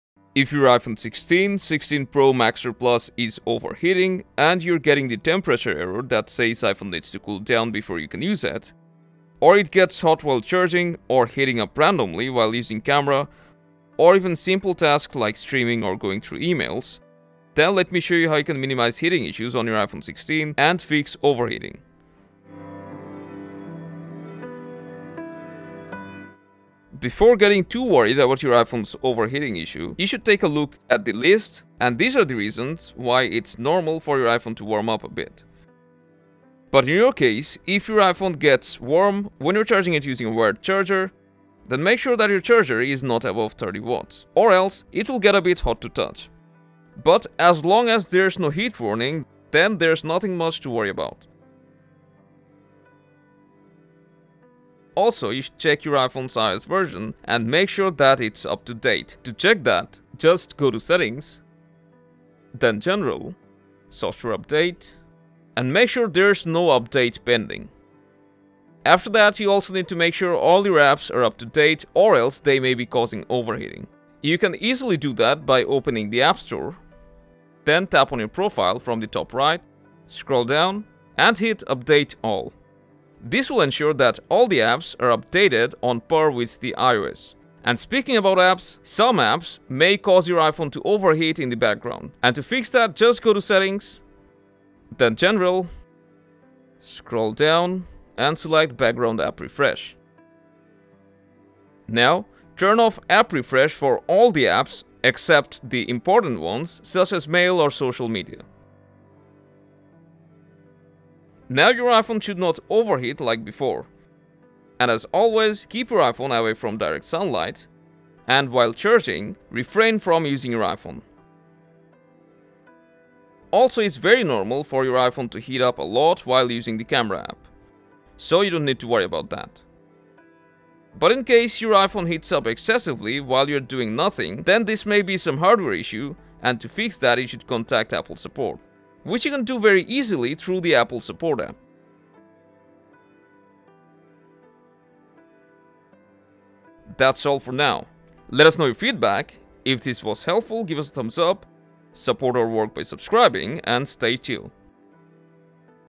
Fix iPhone 16 Pro Max Overheating Issues!
Is your iPhone 16 / 16 Pro Max or Plus overheating and showing the "Temperature, iPhone needs to cool down before you can use it" error? You’re not alone. Many users have been facing similar heating problems with iPhone 16 series iPhones running on the latest iOS 18 while charging or during regular use.

In this video, the Fix369 Team will show you six effective solutions to help resolve overheating issues on iPhone 16 Pro/Max/Plus, which helped us overcome our iPhone 16 Pro Max getting too hot problems.  We’ll also explain why the iPhone 16 can get warm so that you’ll know when it’s normal and when to take action.

0:00 Intro: iPhone 16 Pro Max/Plus Overheating
0:27 Reasons your iPhone could warm up a little
0:38 Solution 1: Use the Correct charging Adopter
0:54 Solution 2: Check iOS update
1:09 Solution 3: Update all Apps
1:38 Solution 4: Background App Refresh
1:54 Solution 5: Cautions to Avoid Overheating
2:11 Solution 6: Apple Support